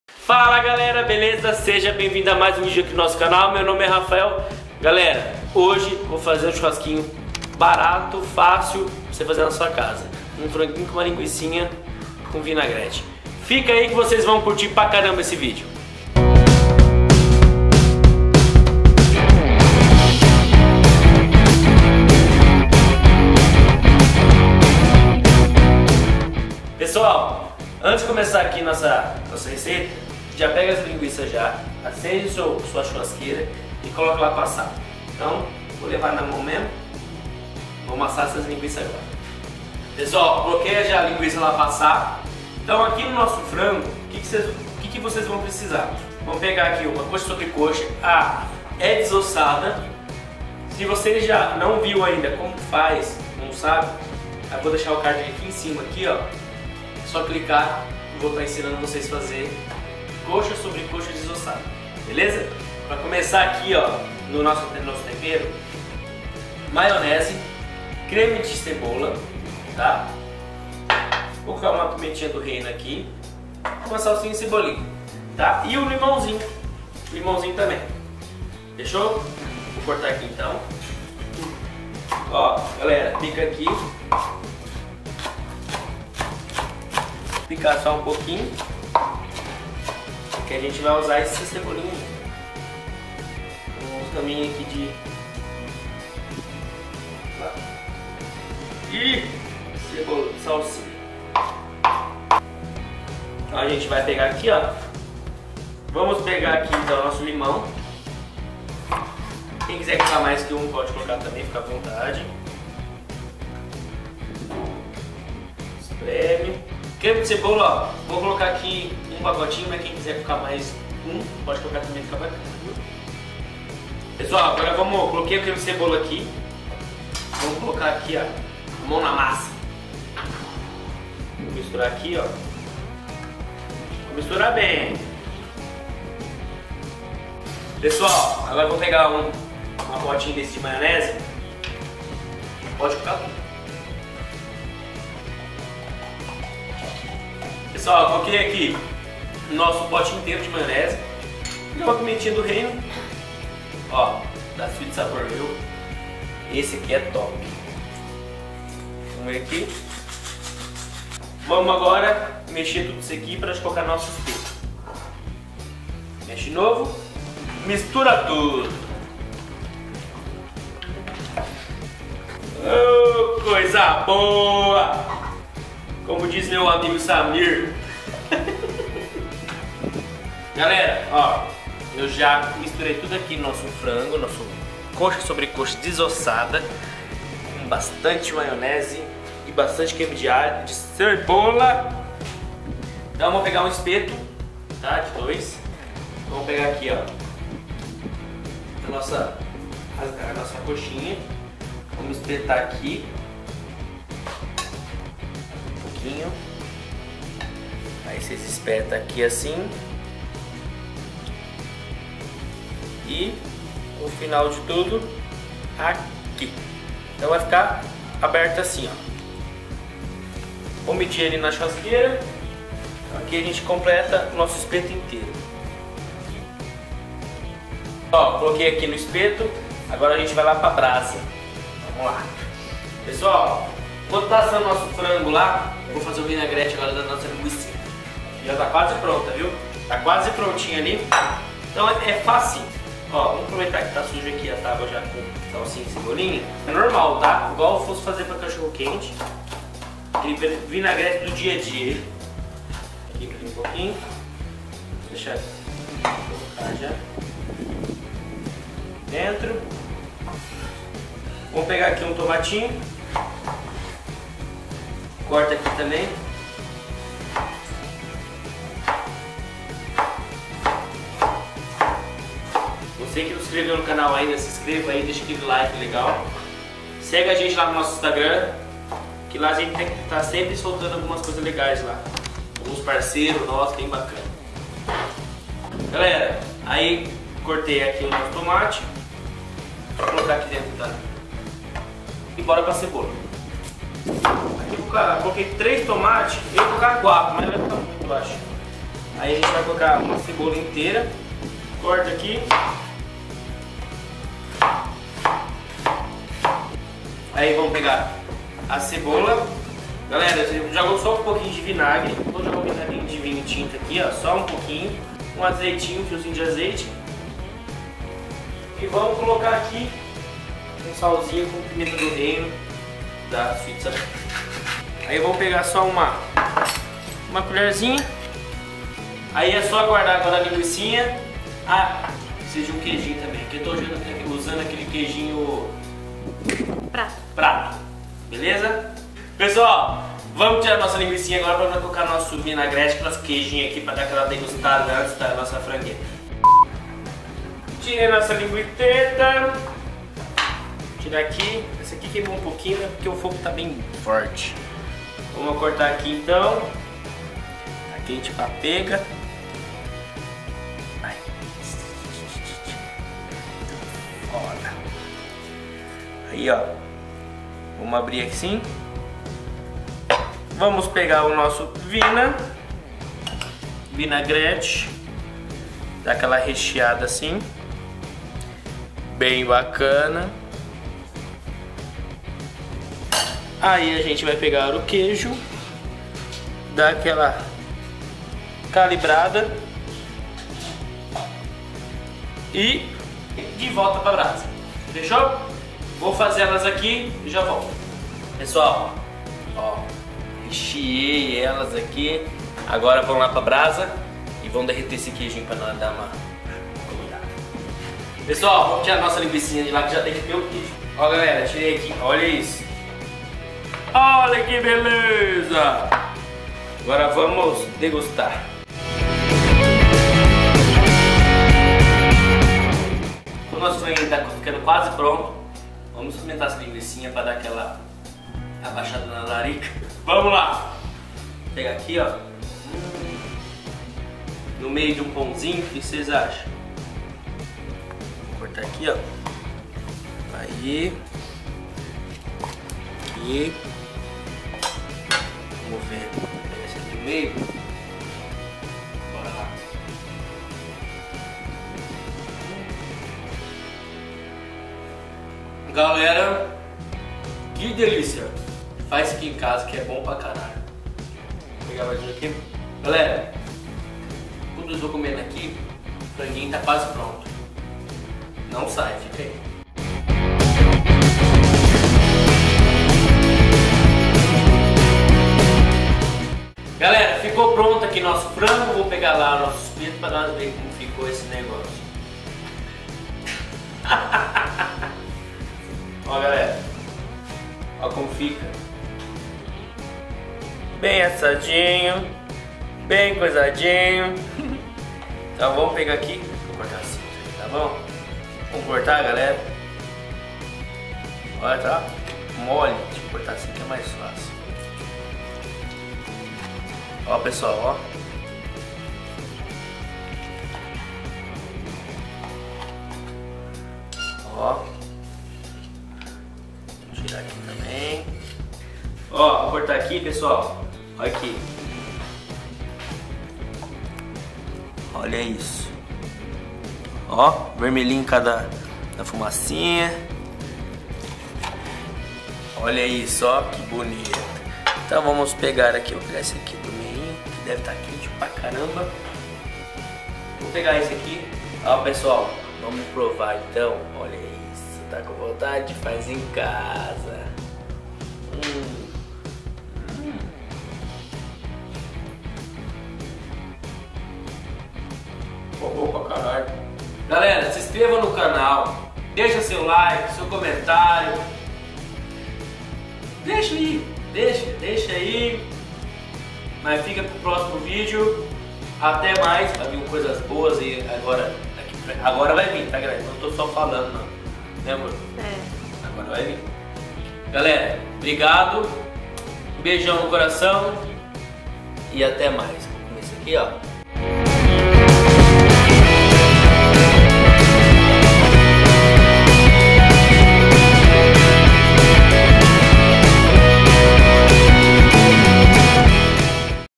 FIZ UM CHURRASCO BARATO E FACIL ***MUITO BOM***
RECEITA - LARICA DO ROLÊ
Com - RAFAEL BRUGIN

● 1kg COXA SOBRE COXA DESOSSADA
● 1 LIMÃO
● 1 CREME DE CEBOLA
● 1 POTE DE MAIONESE
● PIMENTA DO REINO A GOSTO
● SALSINHA E CEBOLINHA

● LINGUIÇA TOSCANA
● 3 TOMATE
● 1 CEBOLA
● VINAGRE A GOSTO
● QUEIJO PRATO RALADO

MODO DE PREPARO:
ASSISTEM O VÍDEO ATÉ O FINAL KKKK.